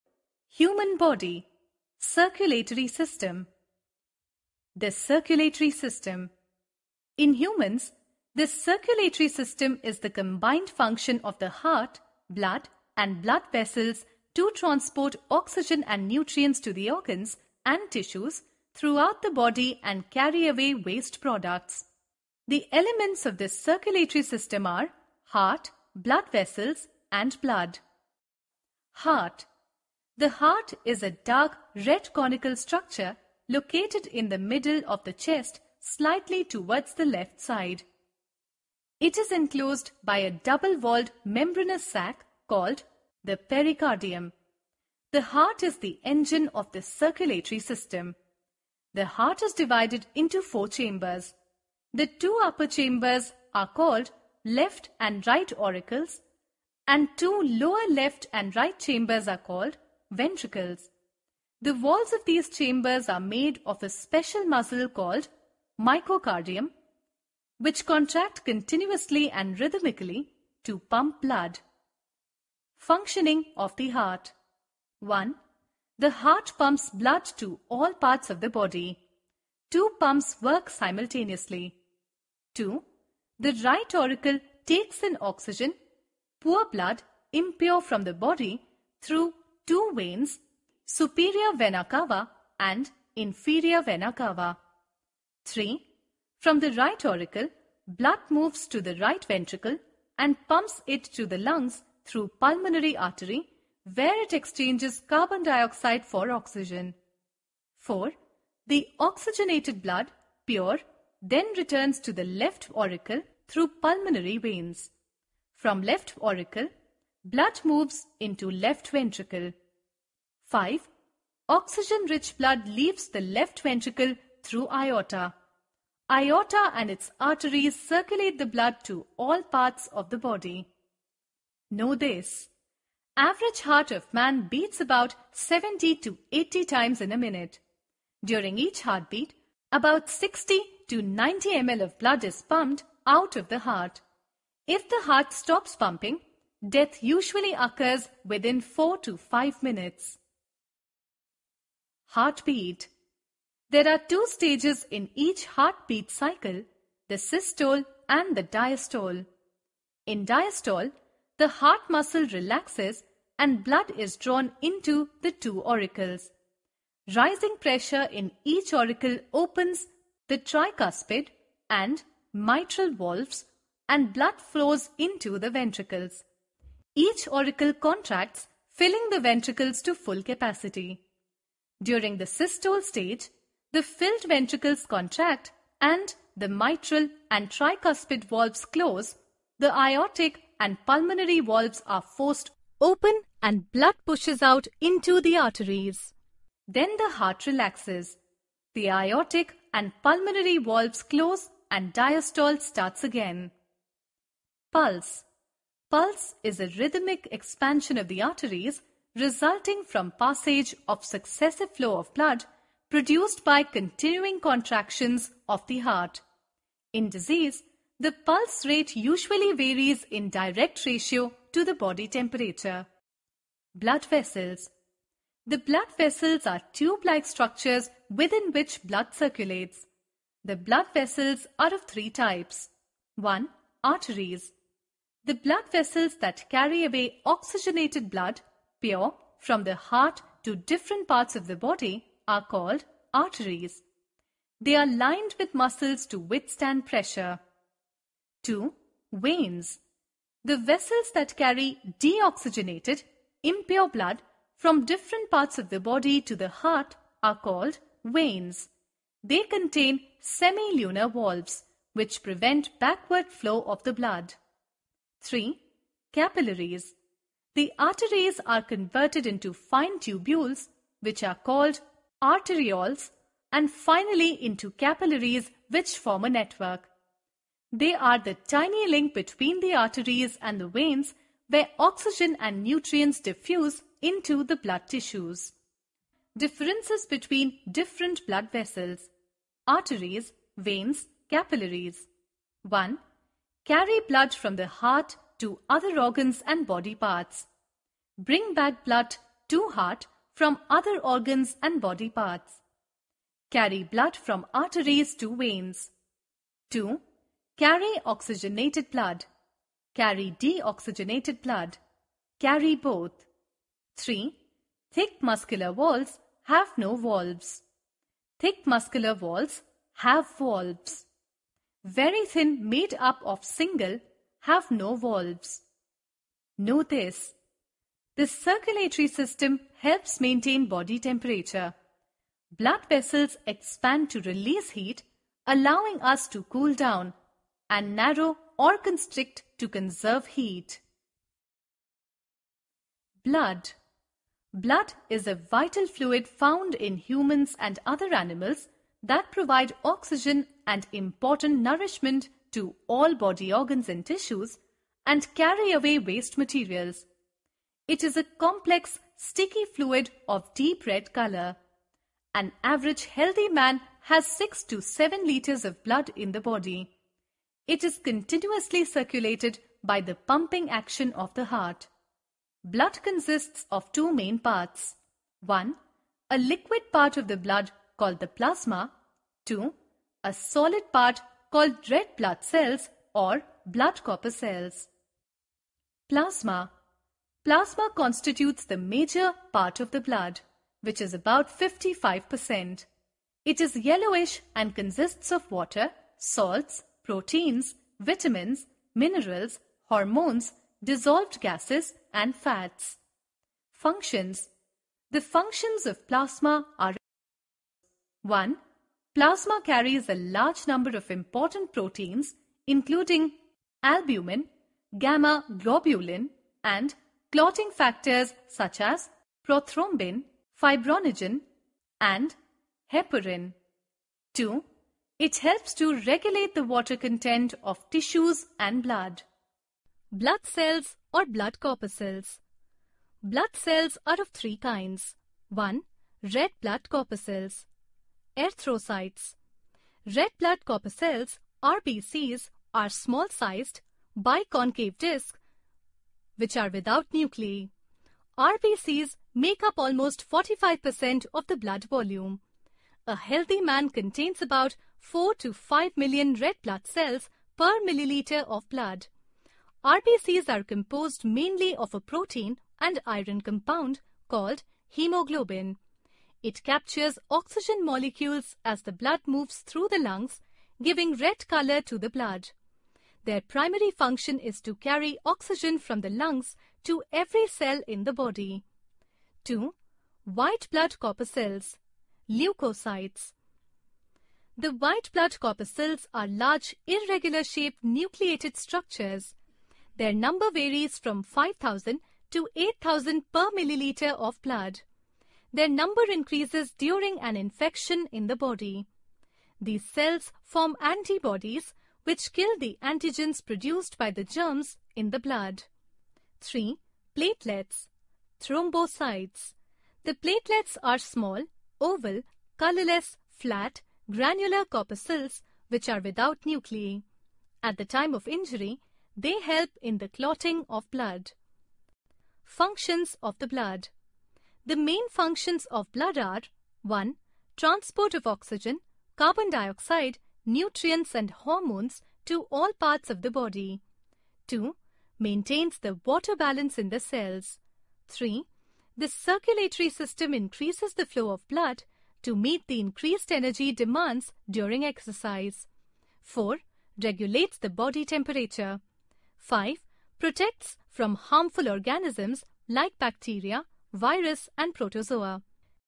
Human Body (Circulatory System) class-8
This video from Kriti Educational Videos explains the Circulatory System of the human body. It explains about the functioning of heart, blood and blood vessel. This video is suitable for class 8.

Contents:
    1. Circulatory System- Introduction
    a. Heart
    b. Blood vessels
    c. Blood